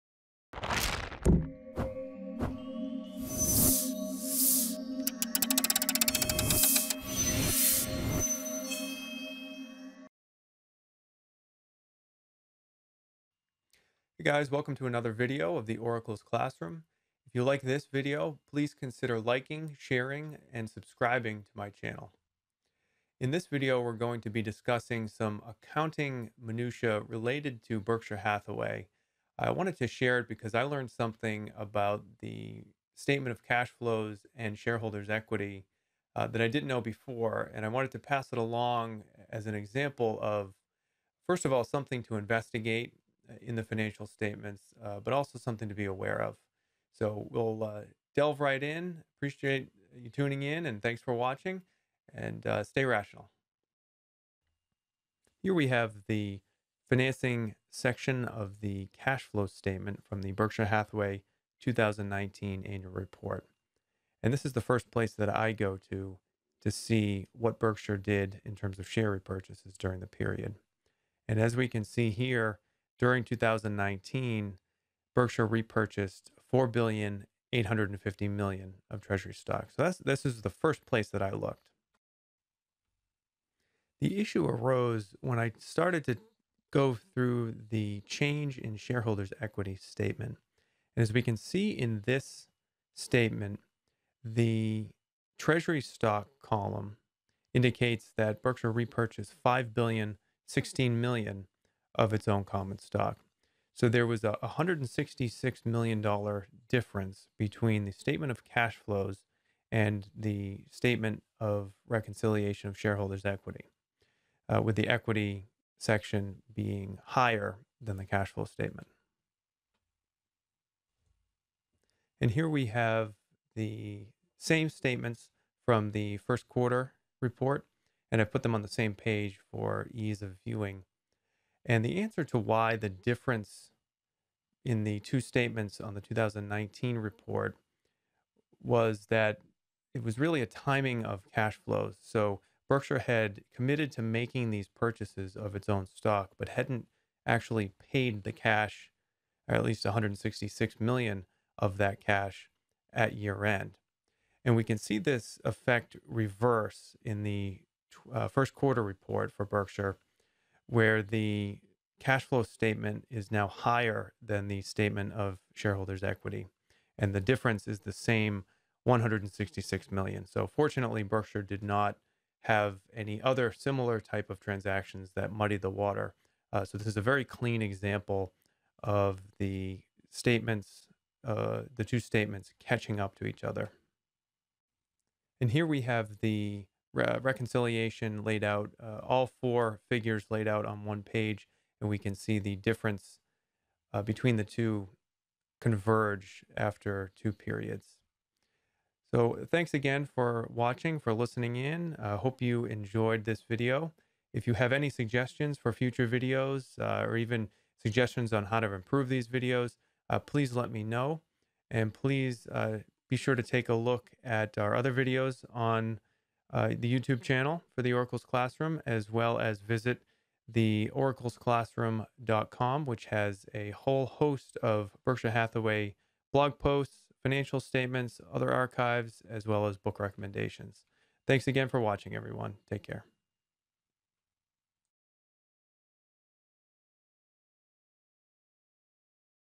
The Case of the Missing $166 Million at Berkshire Hathaway
In 2019, Berkshire Hathaway repurchased $5 billion of its own shares. Or did it?

There was a difference of $166 million between what the statement of shareholders’ equity said the company repurchased and what the statement of cash flows said.

The answer turned out to be very simple, and provided a great accounting lesson.